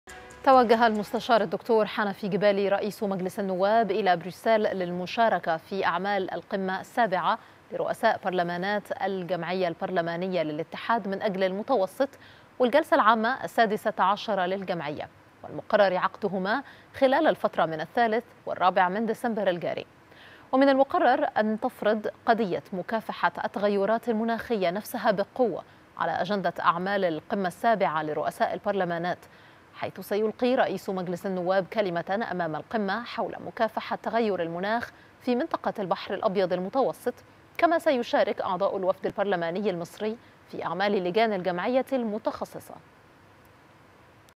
رئيس النواب يتوجه لبروكسل للمشاركة في قمة رؤساء برلمانات الجمعية البرلمانية للاتحاد من أجل المتوسط
غرفة الأخبار| رئيس مجلس النواب يتوجه لبروكسل للمشاركة في قمة رؤساء برلمانات الجمعية البرلمانية للاتحاد من أجل المتوسط

1 ديسمبر 2021